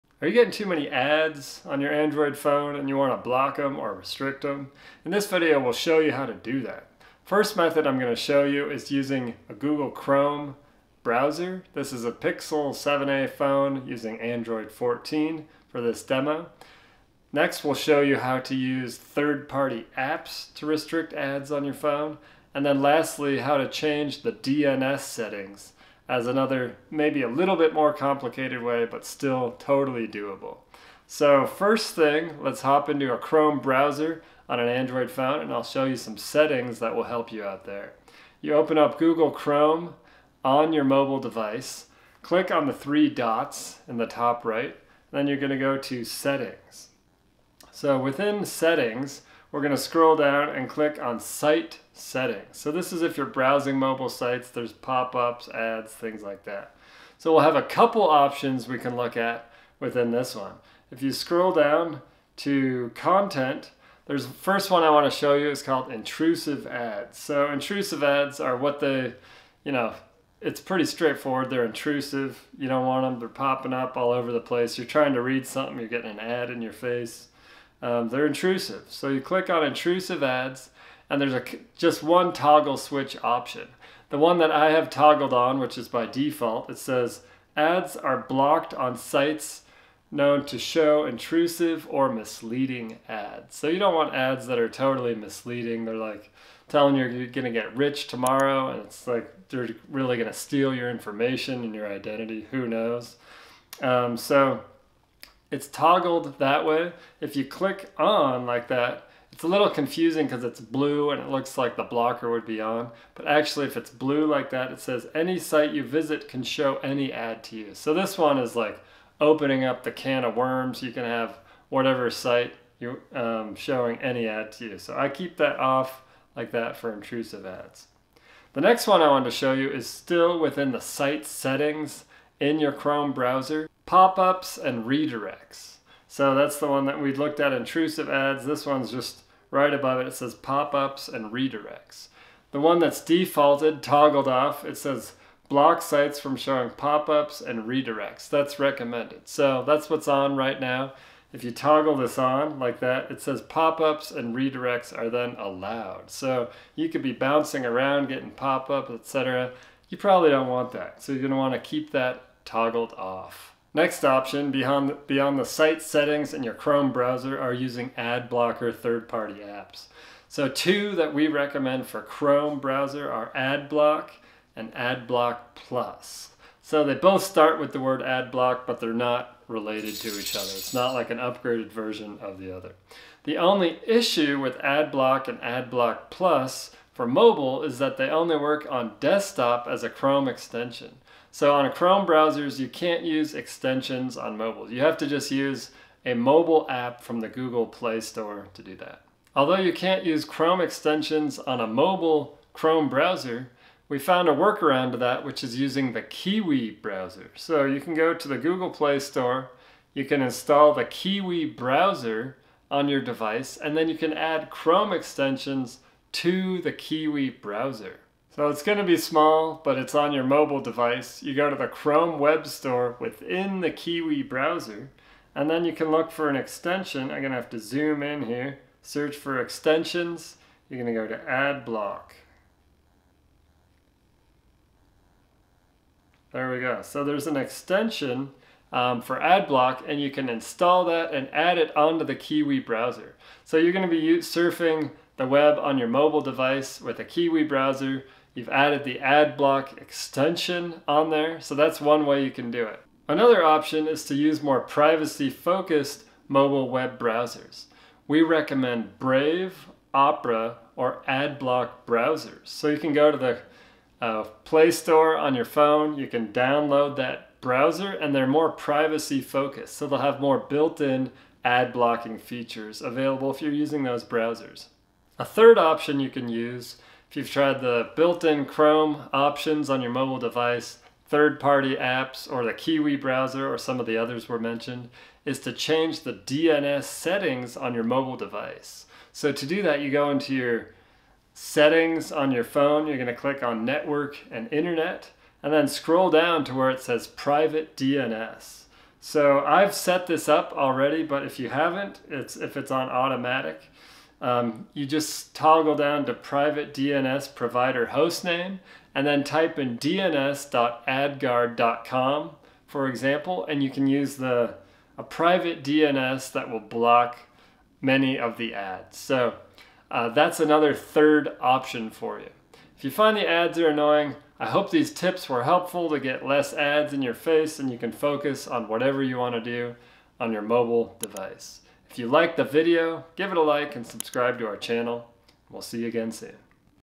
How to Block Ads on Android: Easy Tips and Tricks
Tired of constant ads on your Android phone? In this video, learn three effective methods to block or restrict ads using built-in browser settings, third-party apps, and DNS changes. Follow these steps to enjoy a smoother browsing experience!

📚 Chapters:
0:00 - Introduction
0:31 - Using Google Chrome Settings
2:44 - Third-Party Ad Blockers & Browsers
4:36 - Changing DNS Settings

📋 Methods to Block Ads:
1️⃣ Chrome Browser Settings:
➡️ Open Chrome and tap the three dots.
➡️ Go to "Settings" → "Site Settings" → "Intrusive ads" and make sure it's toggled off.
➡️ In "Site Settings," go to "Pop-ups and Redirects" and make sure it's toggled off.

2️⃣ Third-Party Ad Blockers:
➡️ On PC, install Chrome extensions like Adblock or Adblock Plus.
➡️ Install privacy-focused browsers like Brave, Opera, or Adblock Browser.
➡️ For mobile, use the Kiwi Browser to enable Chrome extensions (like Adblock).

3️⃣ Change DNS Settings:
➡️ Go to "Settings" → "Network & Internet" → "Private DNS."
➡️ Select "Private DNS Provider Hostname" and enter: dns.adguard.com.